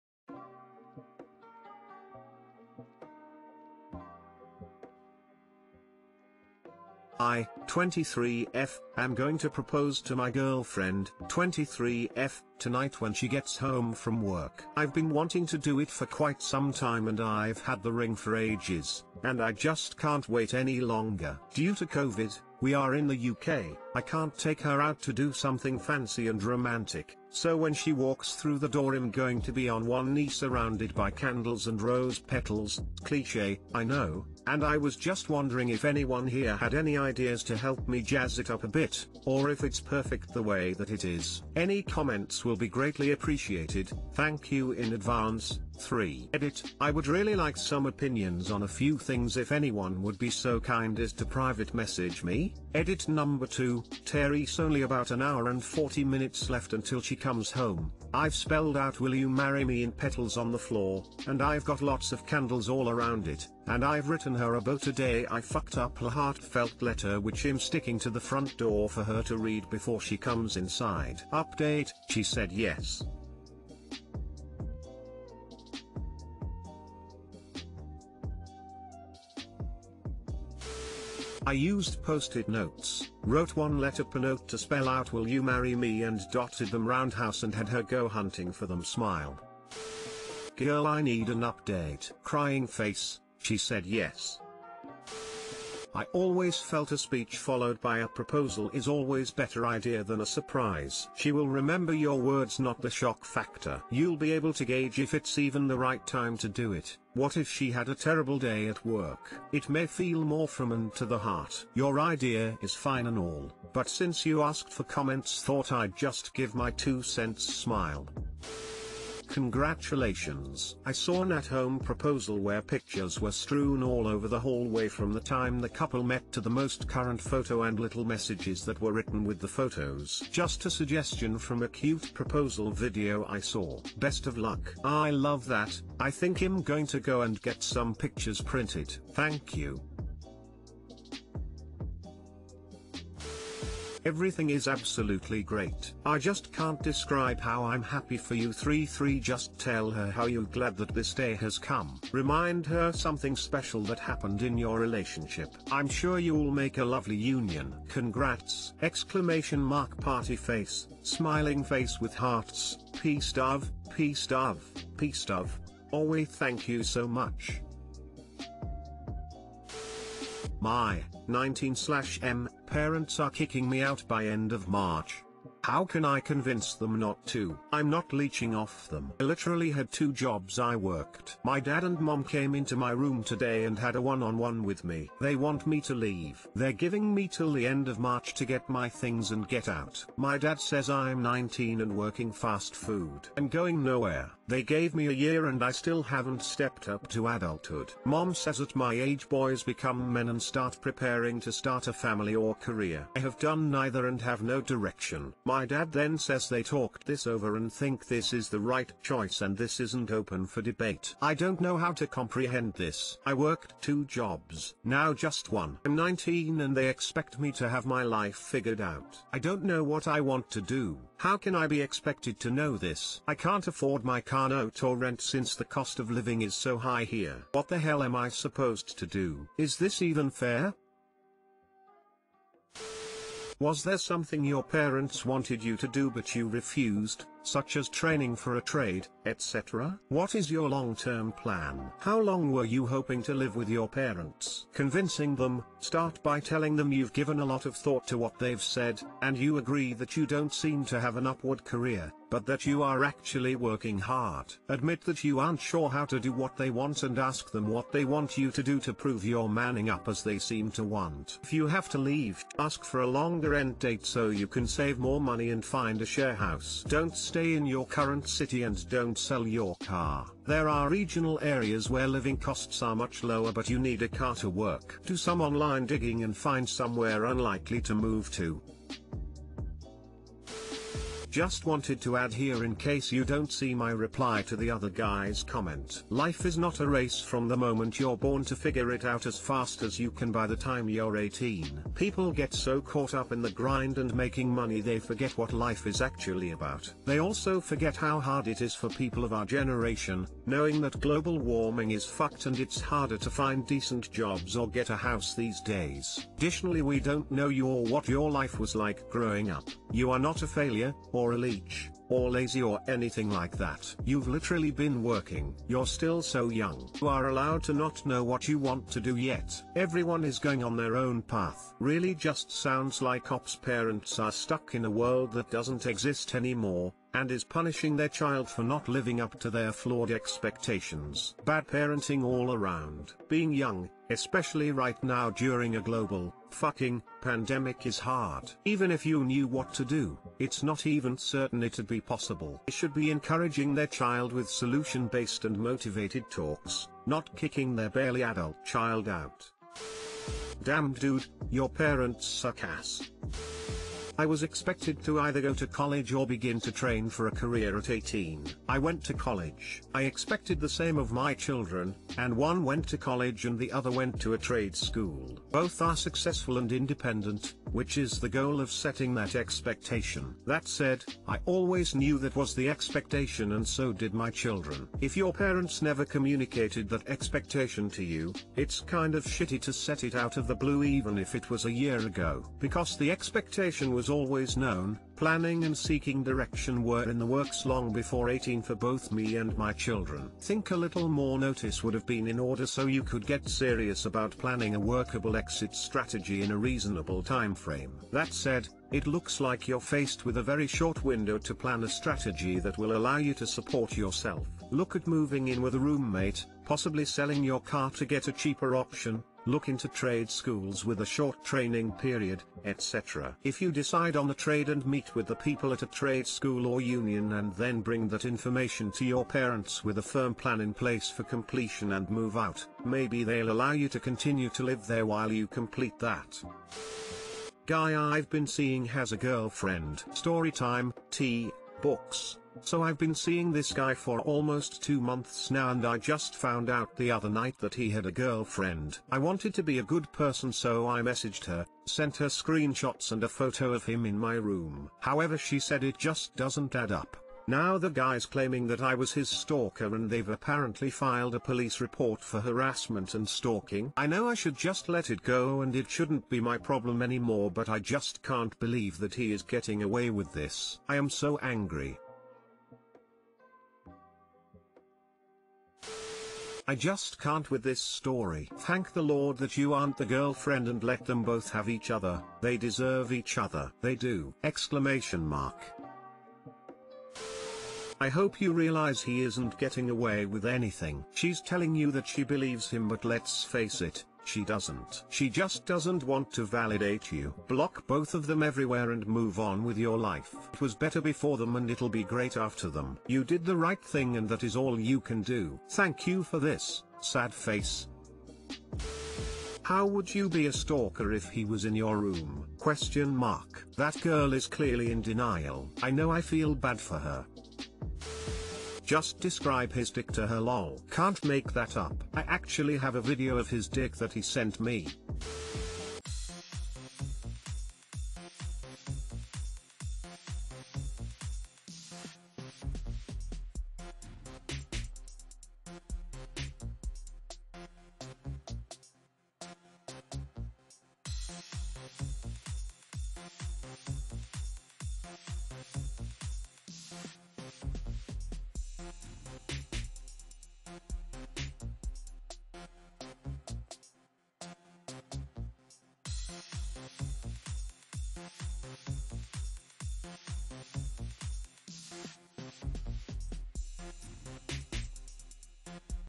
Guy Ive Been Seeing Has A Girlfriend (r/relationships Top Posts | Reddit Stories)
00:07 I am going to propose to my girlfriend tonight when she gets home from work.
02:50 My parents are kicking me out by end of March. How can I convince them not too?
07:07 Guy I’ve been seeing has a girlfriend

I (23F) am going to propose to my girlfriend (23F) tonight when she gets home from work.
My (19/M) parents are kicking me out by end of March. How can I convince them not too?